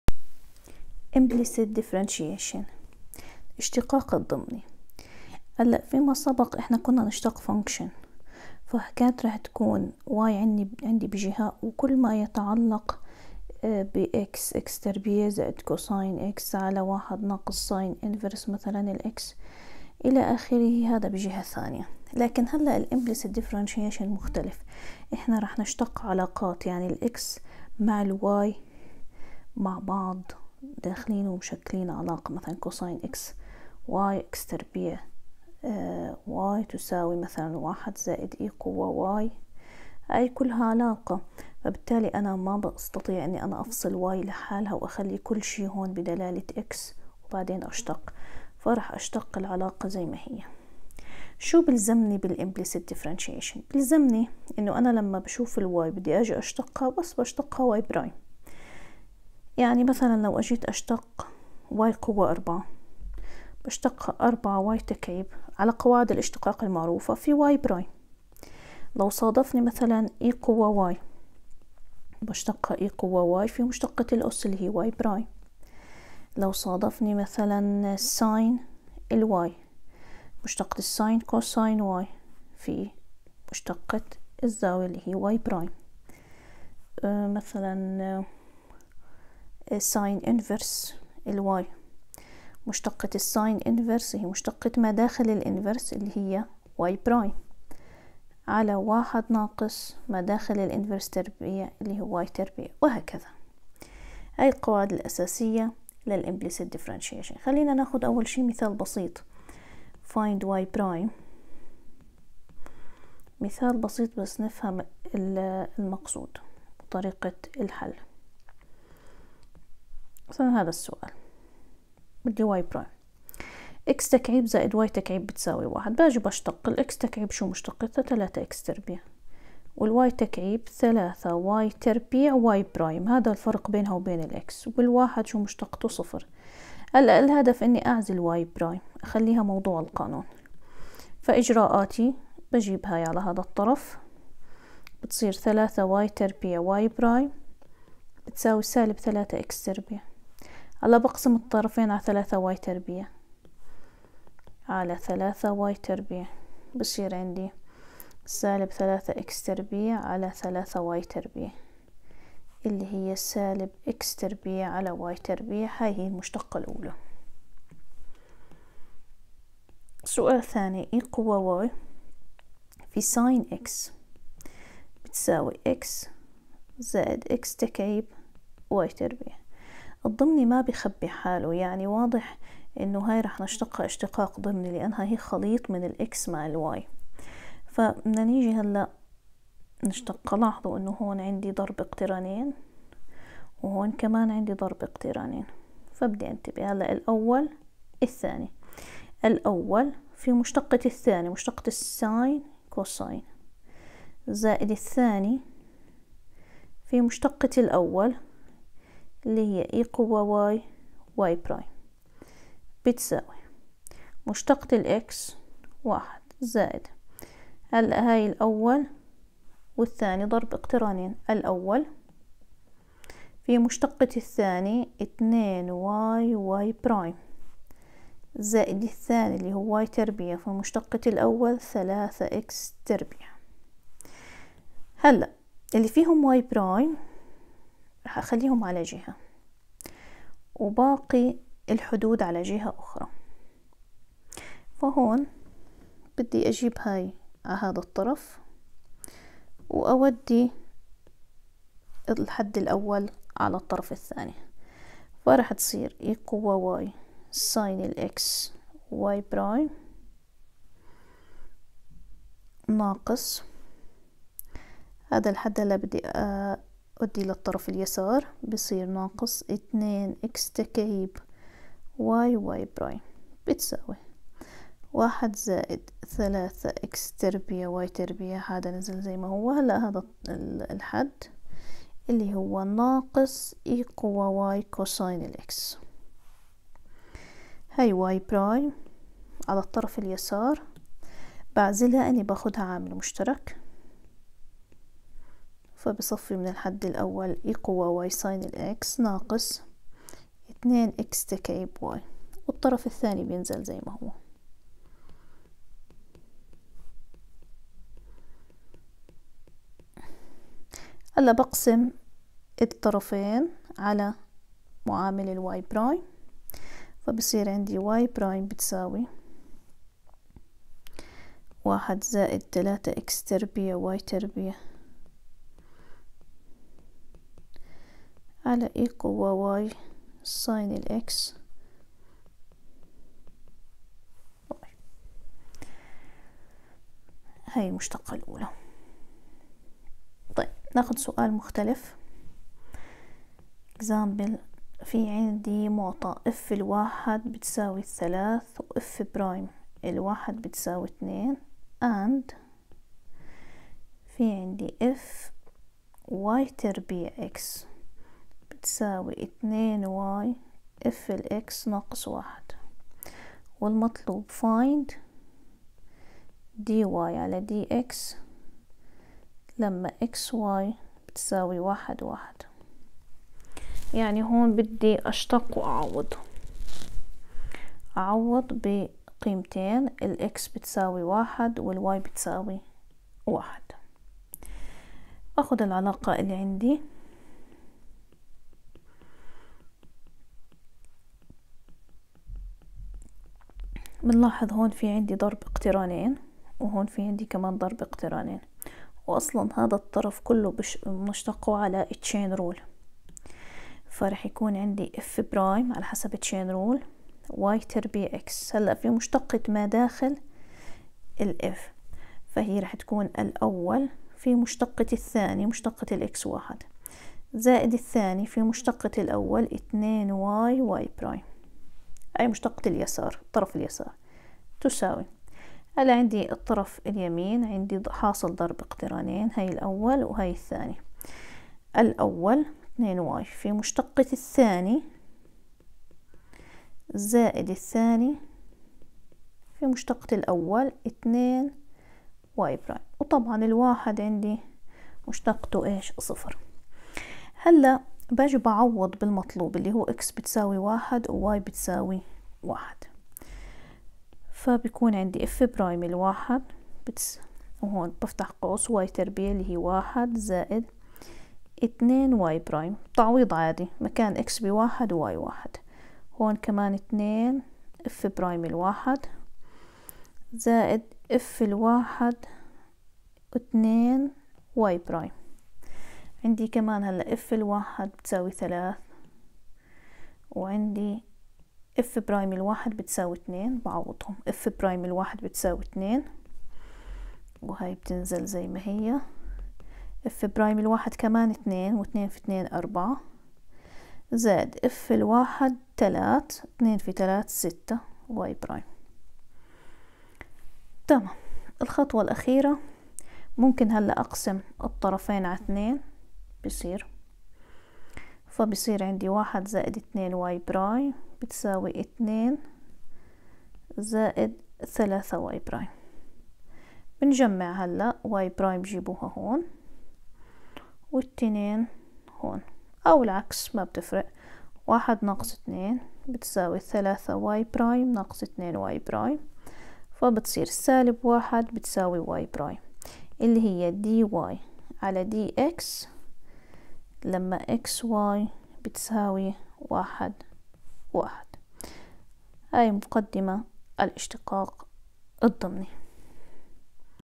Lecture#31(Implicit differentiation)-part#1
how to do implicit differentiation to find dy/dx
استخدام الاشتقاق الضمني لإيجاد المشتقة